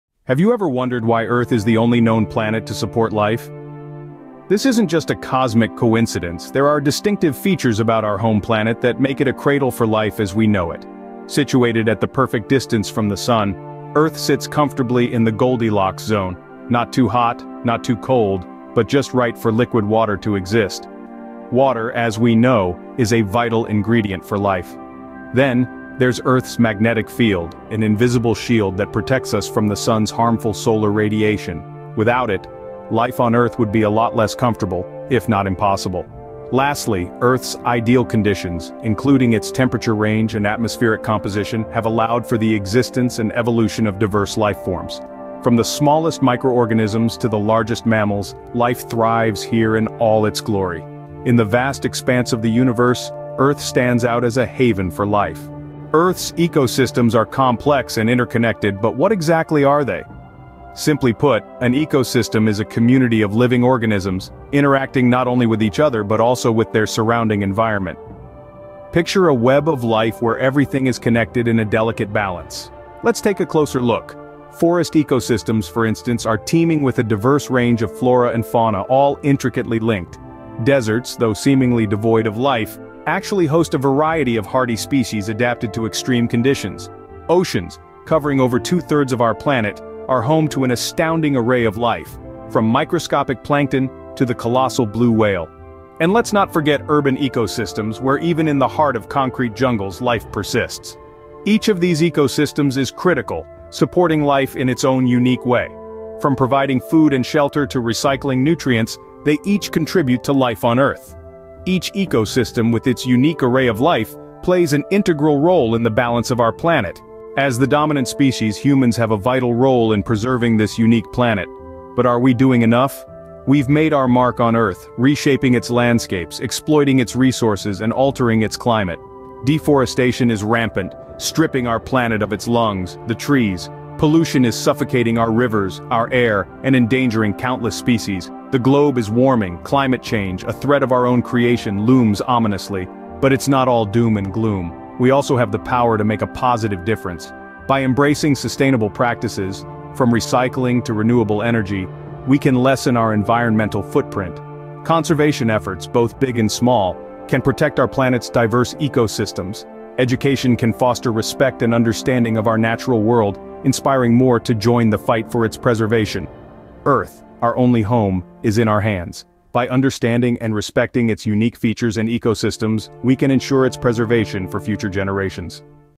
Our Home The Majestic Planet Earth
Explore the wonders of our planet with 'Planet Earth: A Brief Overview'! 🌿 From majestic mountains to vibrant coral reefs, this documentary provides a breathtaking glimpse into the diverse ecosystems that make Earth a truly remarkable place. 🦁 Join us on a journey of discovery as we delve into the beauty, complexity, and fragility of our home. 🌺 Don't miss out on this captivating exploration of nature's wonders! 🌐